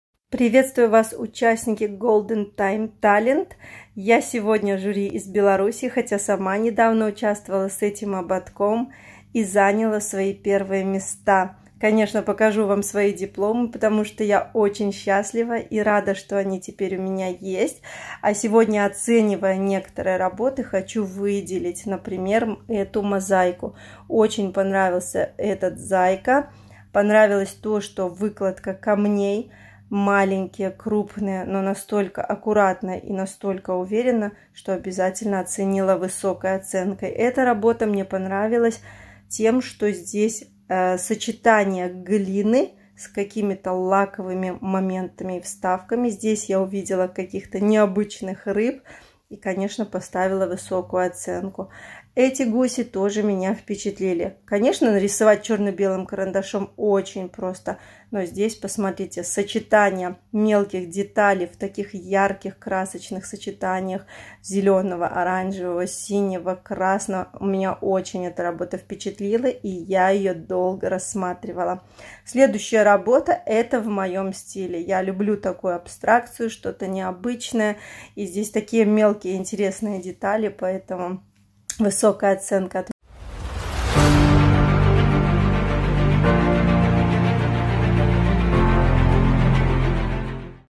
Видео оценка конкурсных работ 22 сезона - жюри конкурса Golden Time Talent - Esipova Ksenia 🇧🇾
Дорогие друзья!
Публикуем видео оценку жюри конкурса Golden Time Talent - Esipova Ksenia 🇧🇾 Belarus